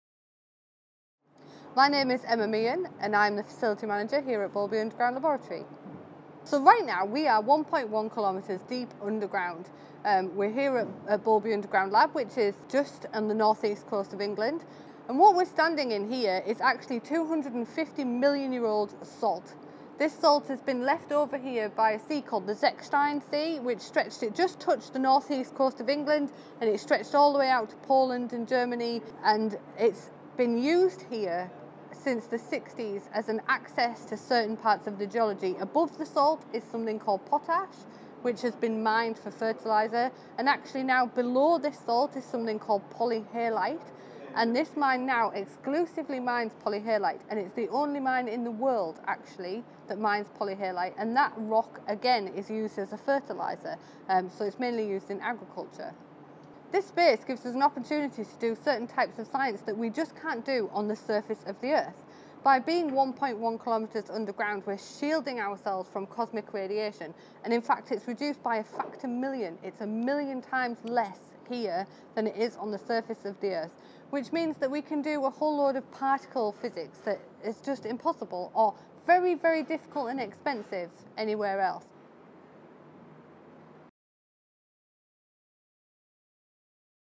Introducing STFC's Boulby Underground Laboratory
Boulby Underground Laboratory is the UK’s deep underground science facility, located 1.1km below ground in Boulby mine, a working potash, polyhalite and salt mine in the North East of England.

Find out more about this amazing facility and the science that goes on here with Facility Manager, Emma Meehan.